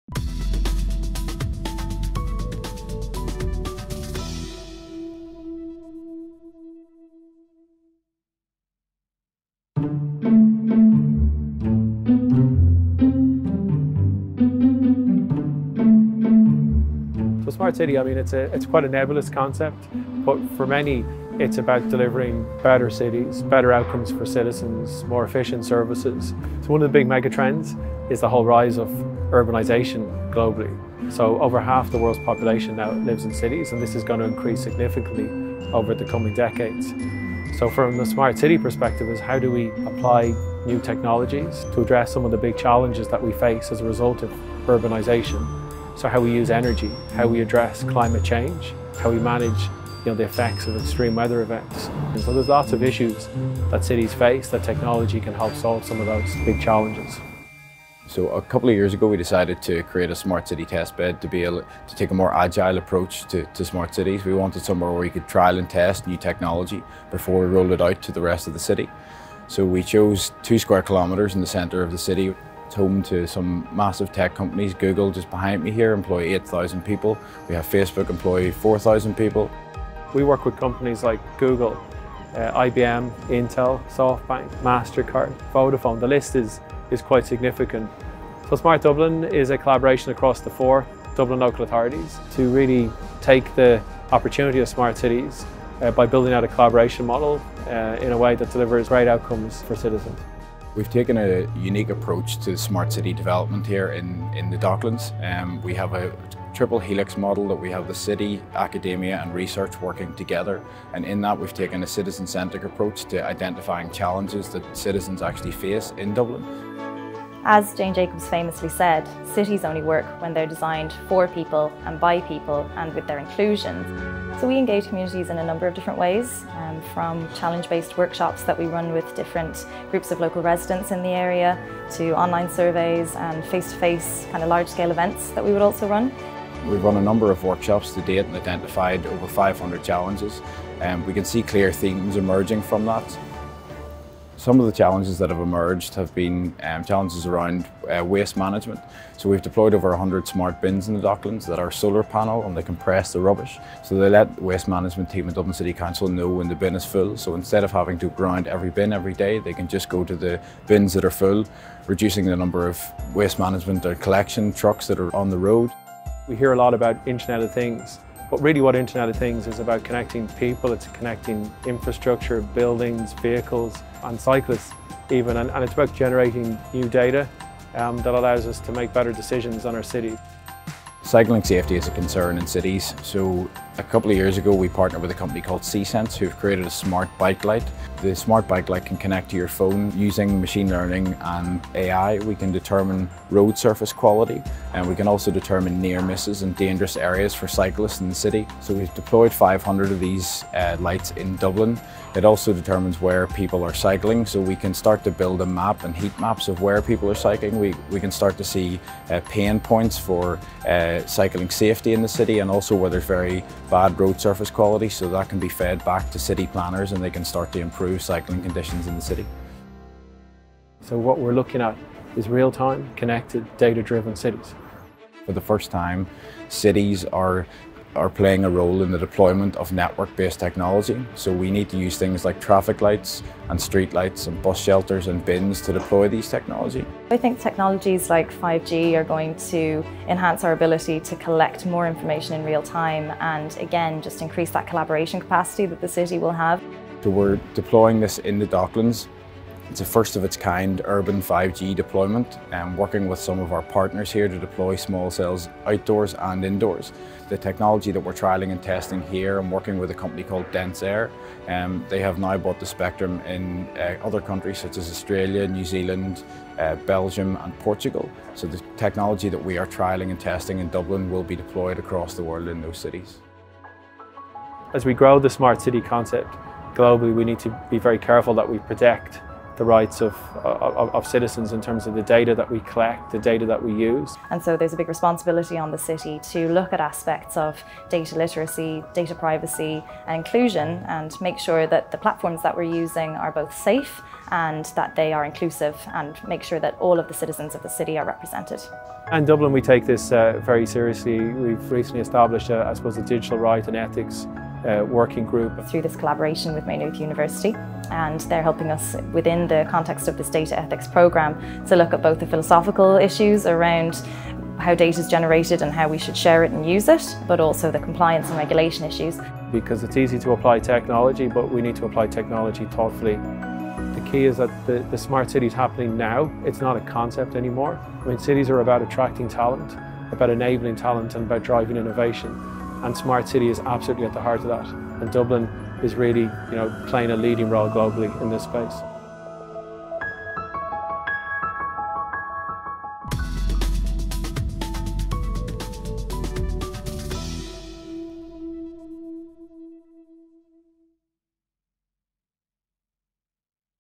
Dublin, a City Getting Smarter
Urbanisation one of the big megatrend of our time with over half of the world's population now living in cities. Cities are facing serious challenges such as climate change, extreme weather events, traffic congestion and environmental pressure. However the rise of Internet of Things, Big Data, Cloud Computing, Socialand Mobile presents real opportunities to apply these new technologies in a way that can make cities more sustainable, efficient and engaging for residents and visitors alike. With the creation of the Smart Dublin programme in 2016 the city took a significant step towards turning its challenges into opportunities for innovation. Dublin is now leading the way through its collaboration model in how it is bringing together industry, tech companies, start-ups, academia and citizens to realise the smart city opportunity and fast track progress with the creation of its Smart Docklands district.